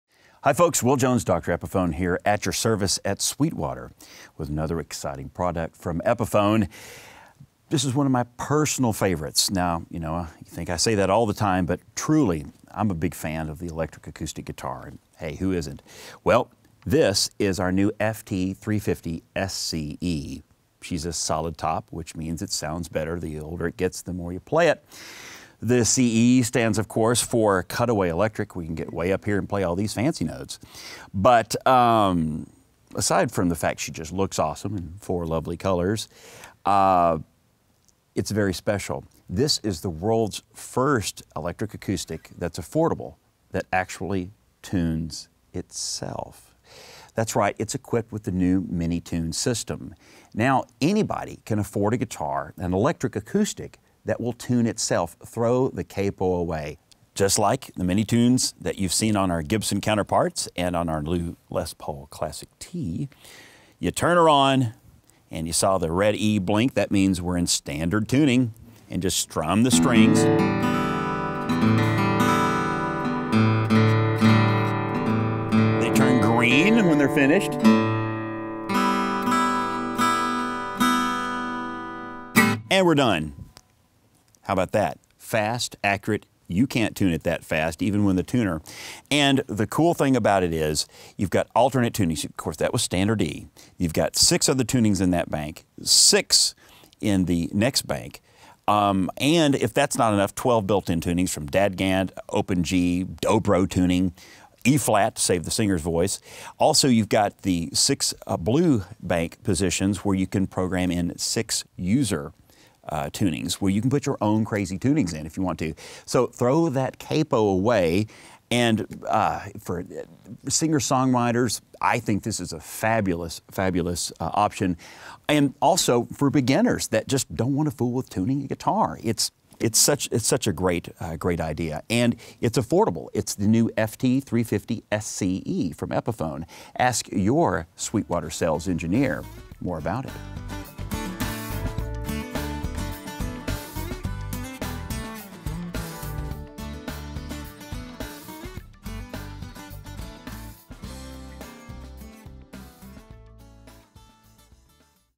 Sweetwater's Epiphone FT-350SCE with Min-ETune Guitar Demo
Dr. Epiphone, AKA Will Jones, presents the Epiphone FT-350SCE acoustic-electric guitar with Min-ETune auto-tuning system. After the video, click the link above for more info and hi-res photos in the Sweetwater Guitar Gallery!